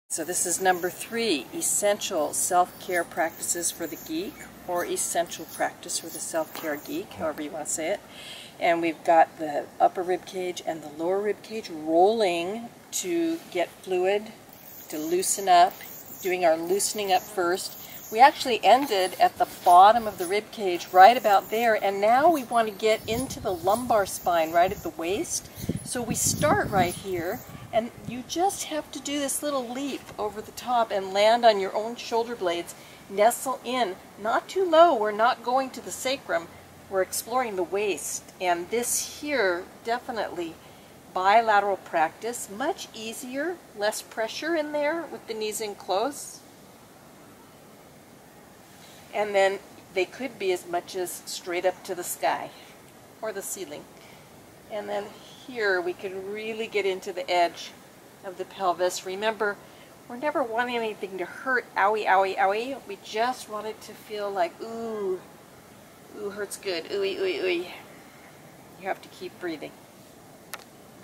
#3 Essential Practice for the Self Care Geek
#3 Rolling the tender lumbar waist. Spontaneously filmed MacKerriker Beach, May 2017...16 essential practices, the foundational instruction offered in Tuesday morning Yoga Therapy. info